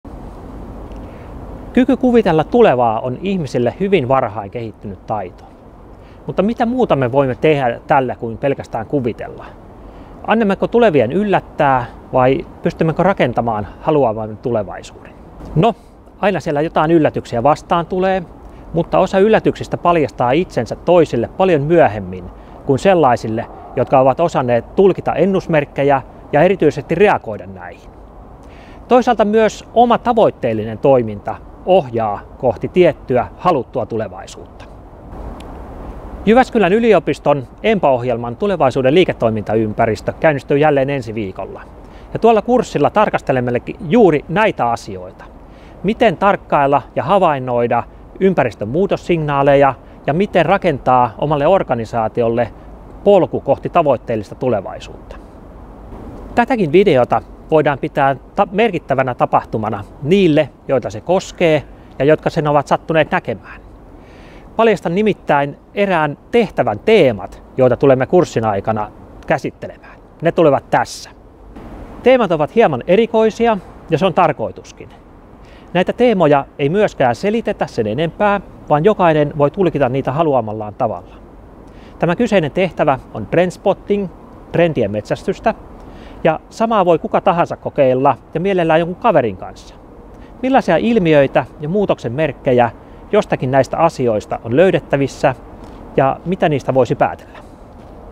Tulevaisuuden liiketoimintaympäristö
jyuEMBA:n valinnaisiin opintojaksoihin kuuluva tulevaisuuden liiketoimintaympäristö -kurssin teemaennakko.